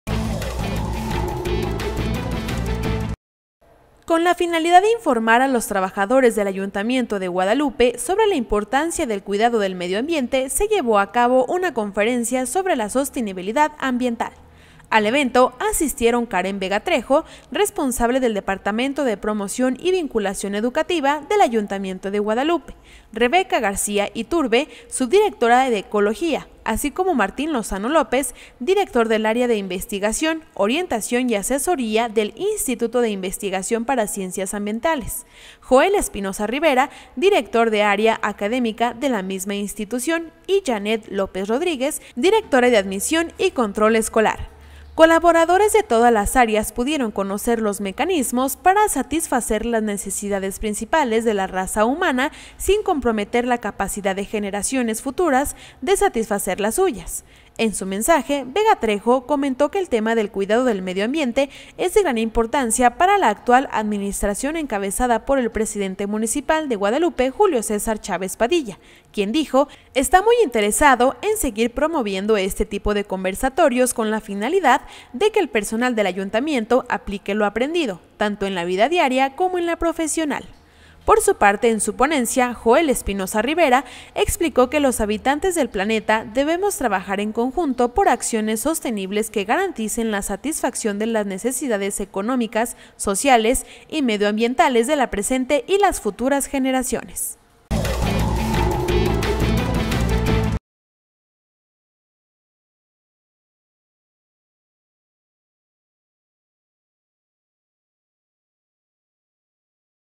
Imparten conferencia de Sostenibilidad Ambiental en Ayuntamiento de Guadalupe
Con la finalidad de informar a los trabajadores del Ayuntamiento de Guadalupe sobre la importancia del cuidado del medio ambiente, se llevó a cabo una conferencia sobre la Sostenibilidad Ambiental. Al evento asistieron Karen Vega Trejo, responsable del Departamento de Promoción y Vinculación Educativa del Ayuntamiento de Guadalupe; Rebeca García Iturbe, subdirectora de Ecología; así como Martín Lozano López, Director del Área de Investigación, Orientación y Asesoría del Instituto de Investigación para Ciencias Ambientales; Joel Espinoza Rivera, Director de Área Académica de la misma institución y Janet López Rodríguez, Directora de Admisión y Control Escolar